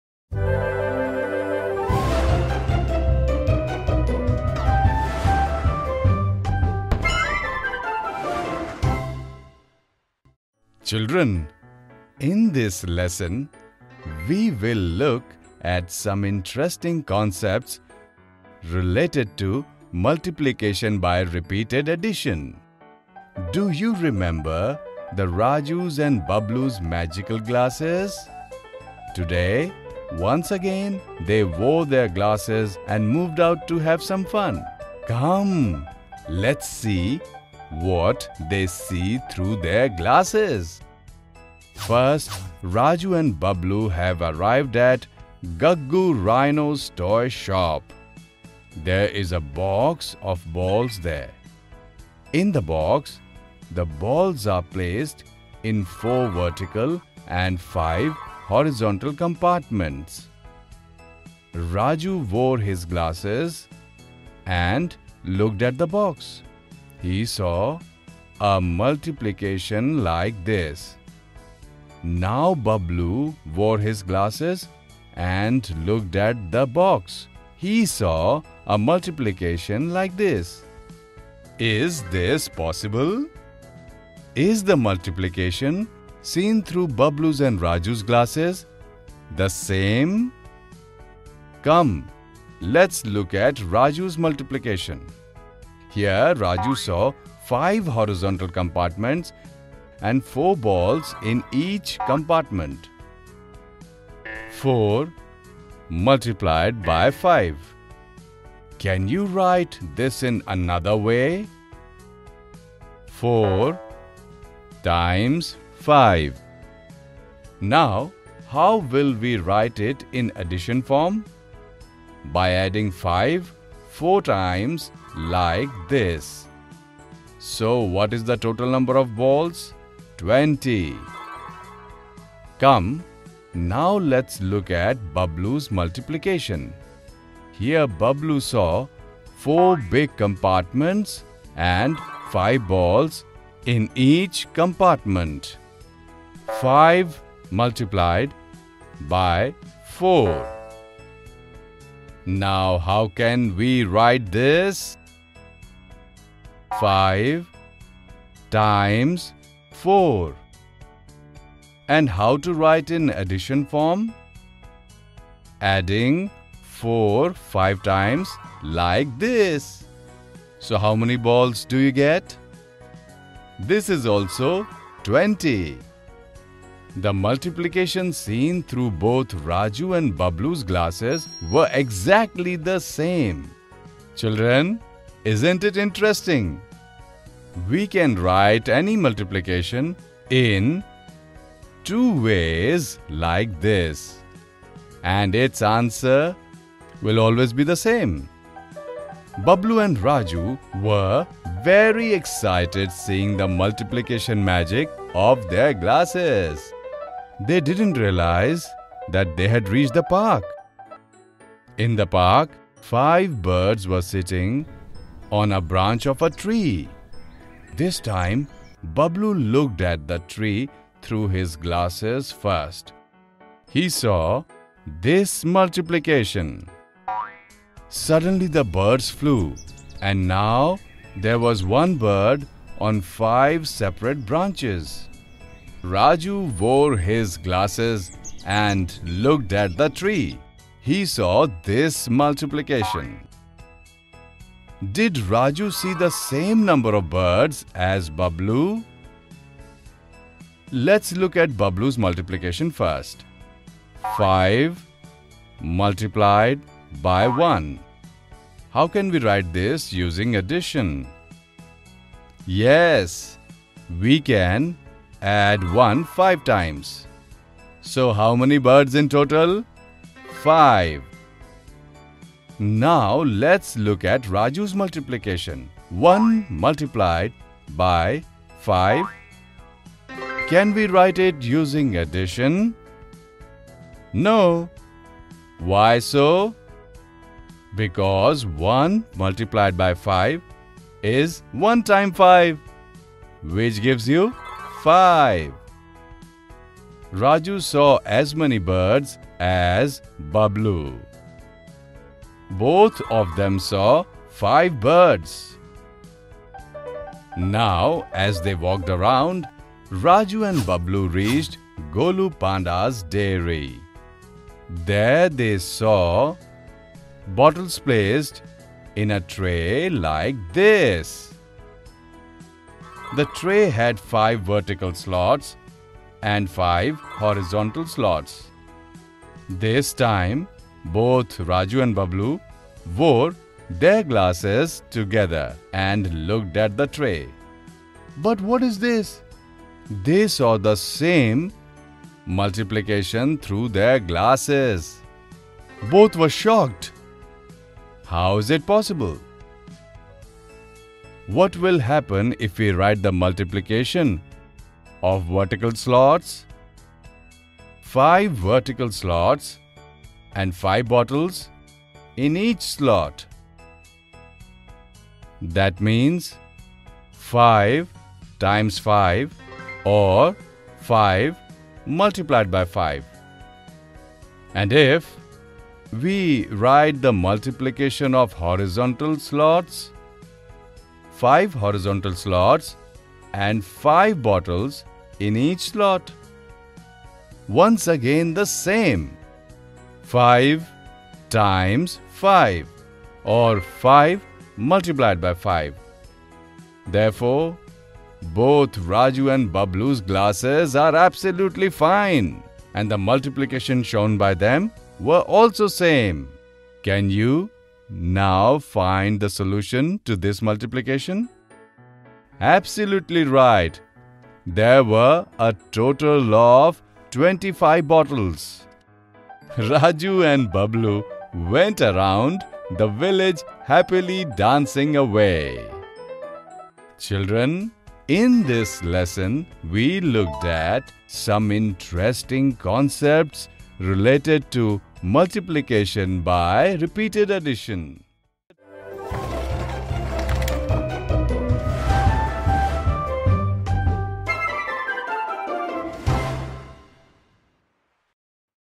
Multiplication as repeated addition | Part 3/3 | English | Class 3
Multiplication as repeated addition | Part 3/3 | English | Class 3 |  | NCERT |

Multiplication as repeated addition | Multiplication and Division | Grade 3 | NCERT | How many times

Welcome to part 3 of Multiplication as repeated addition. In this video, we will discuss various misconceptions and/or common errors related to Multiplication as repeated addition.